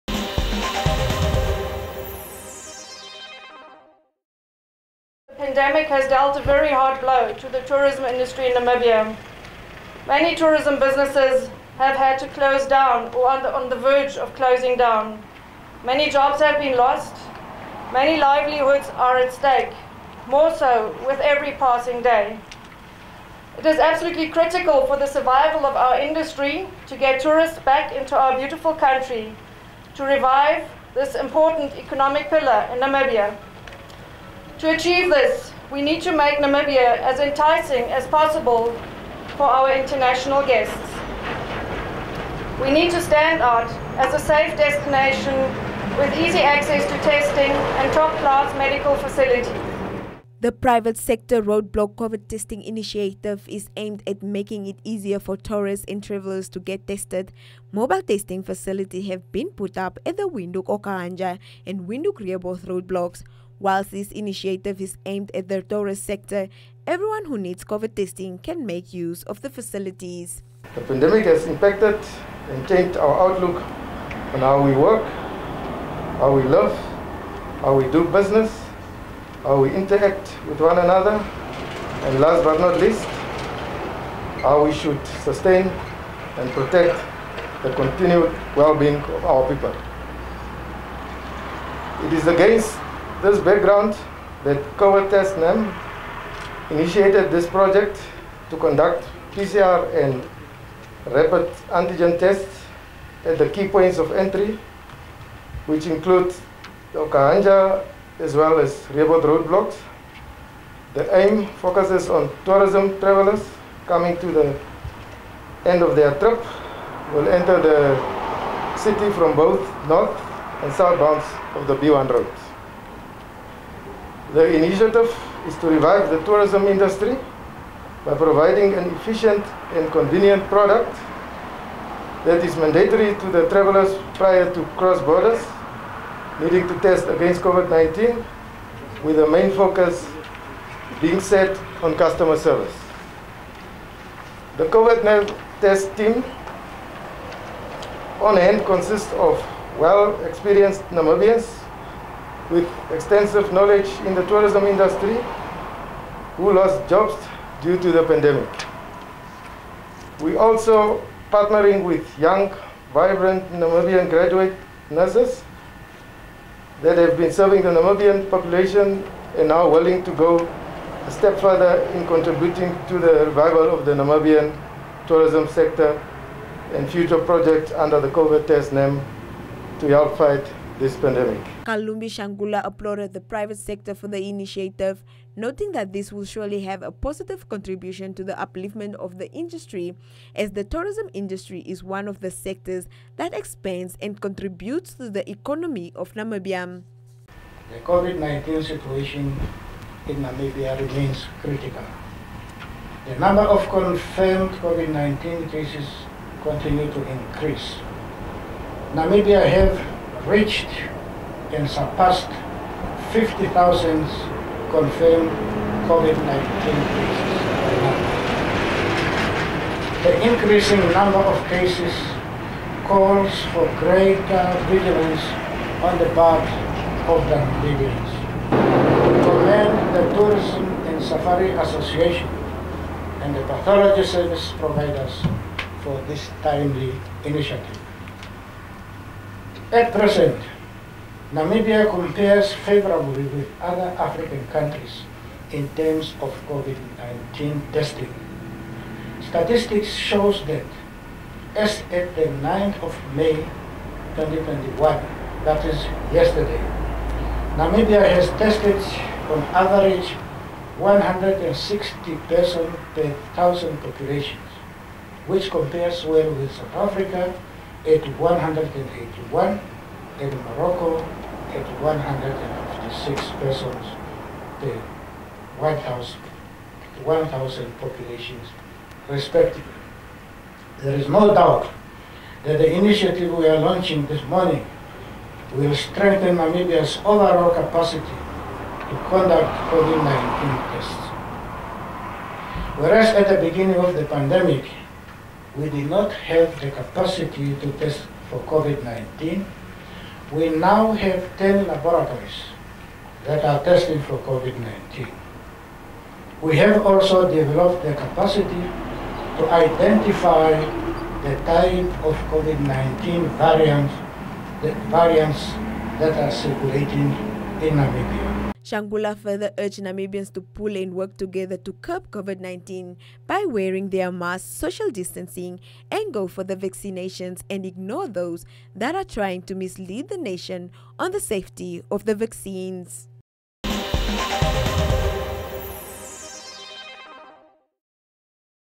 Private sector roadblock COVID testing initiative launched
In making COVID testing more accessible for international travellers, the Tour and Safari Association of Namibia in collaboration with Diagnolab & HT Trading Solutions, with the support from the Hospitality Association of Namibia (HAN) joined forces by launching the private sector road-block COVID testing initiative at roadblocks such as Windhoek/Okahandja and Windhoek/Rehoboth.